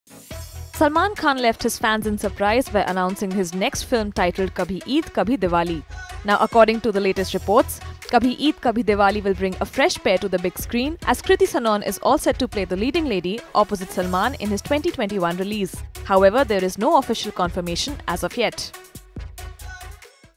'Kabhi Eid Kabhi Diwali': Kriti Sanon cast opposite Salman?
Salman Khan left his fans in surprise by announcing his next film titled 'Kabhi Eid Kabhi Diwali'. Now according to the latest reports, 'Kabhi Eid Kabhi Diwali' will bring a fresh pair to the big screen as Kriti Sanon is all set to play the leading lady opposite Salman in his 2021 release. However, there is no official confirmation as of yet.